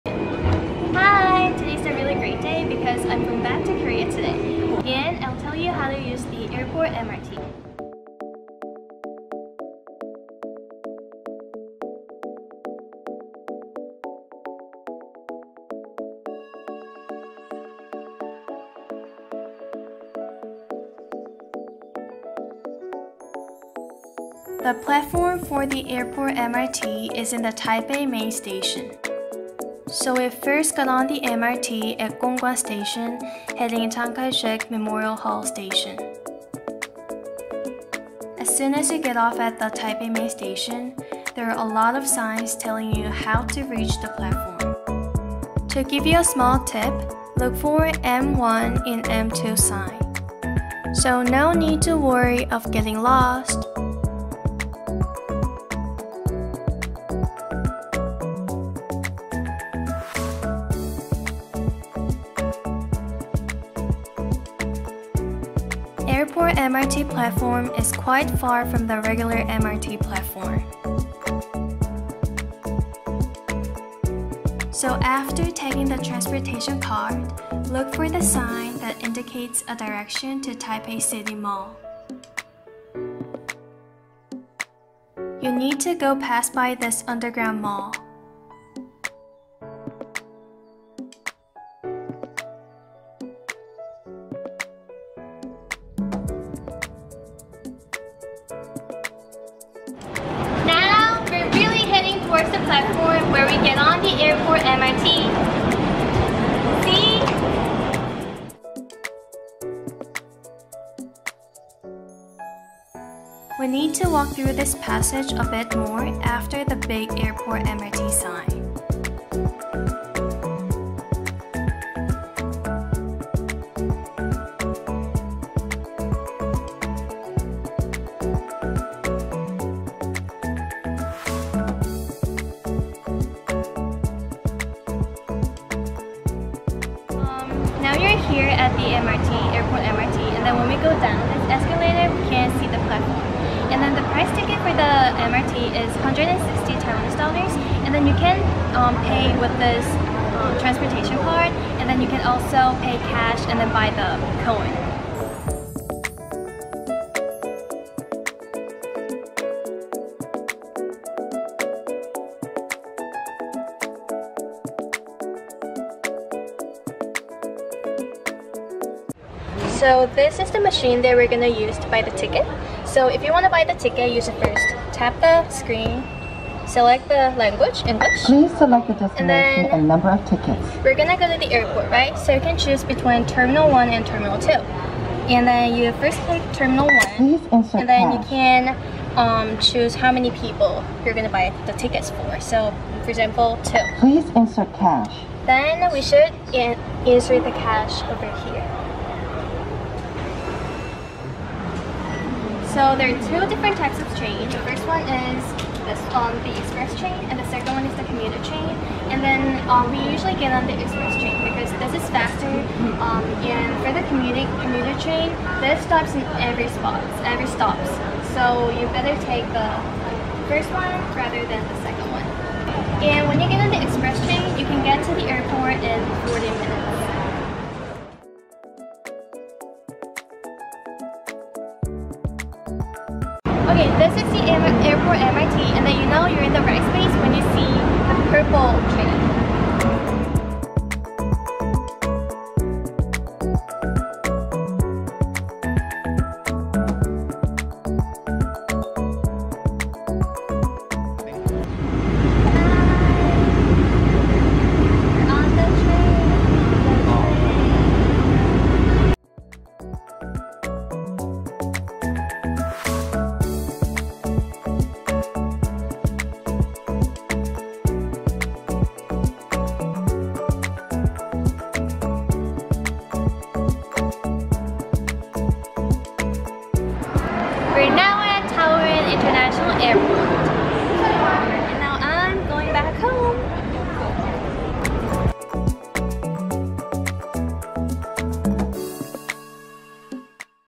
Taipei Taoyuan Airport MRT l How to get to the airport l 機場捷運
This time, we filmed a video about the Taipei Airport MRT!!
You can get on the Taipei Airport Metro at Taipei Main Station and it is 160NTD(one way).
From Taipei Main Station, it only takes about 40 minutes to get to the Taoyuan International Airport.